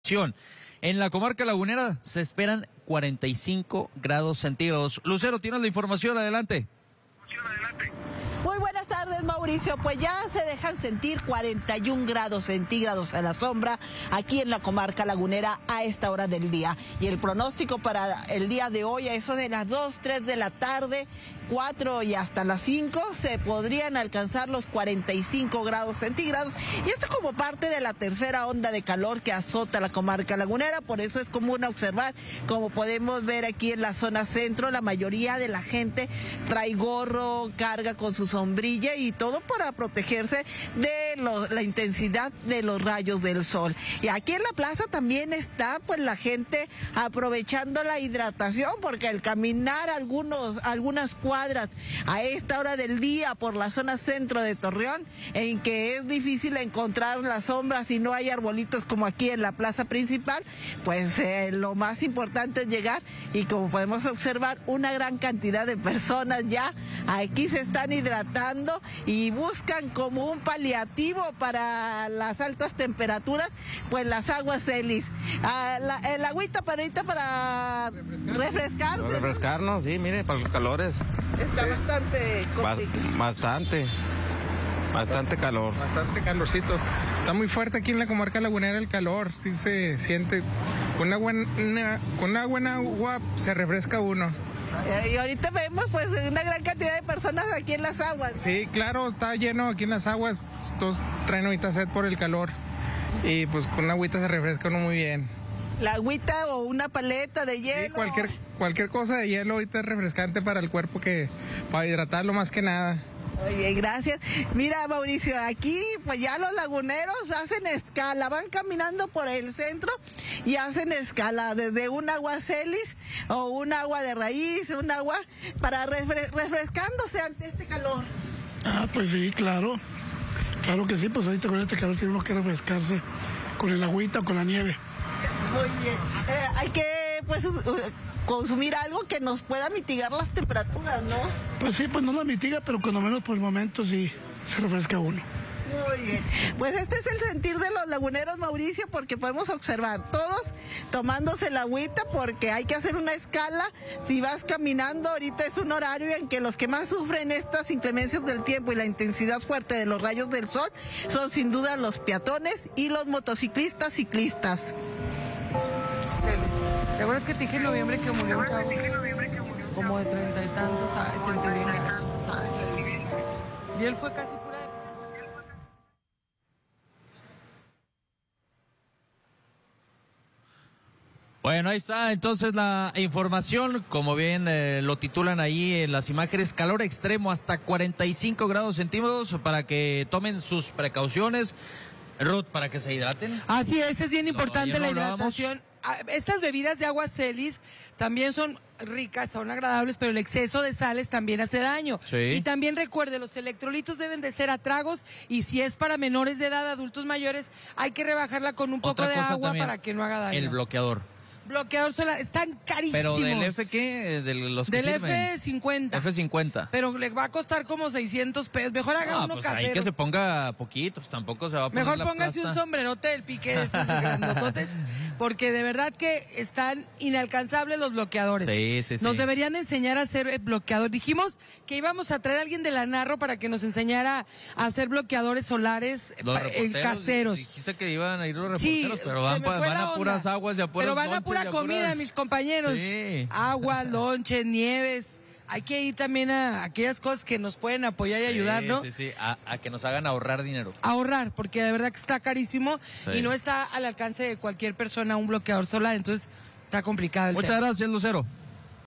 Pronostican temperaturas de hasta 45 grados en La Laguna
El calor no da tregua a los laguneros, pues los más recientes pronósticos indican que durante los próximos días se podrían registrar hasta 45 grados centígrados.